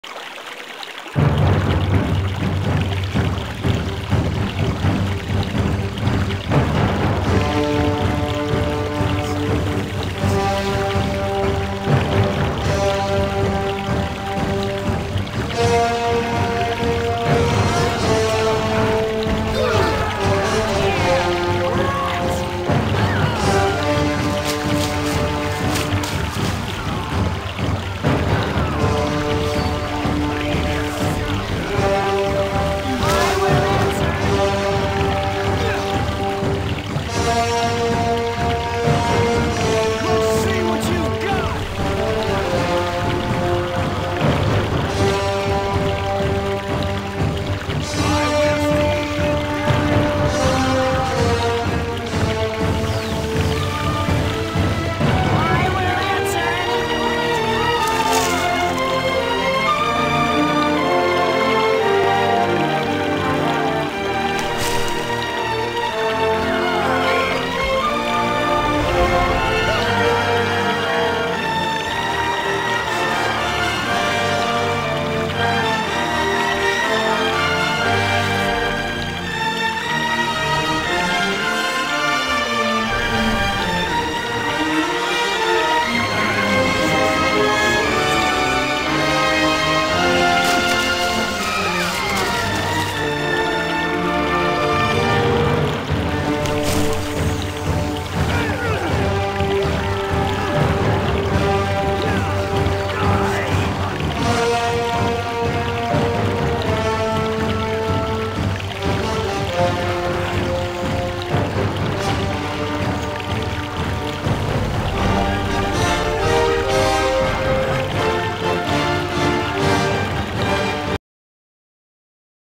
Conan in Bossonian Marches
Conan and his band of mercenaries battle Nemedians in the Bossonian Marches.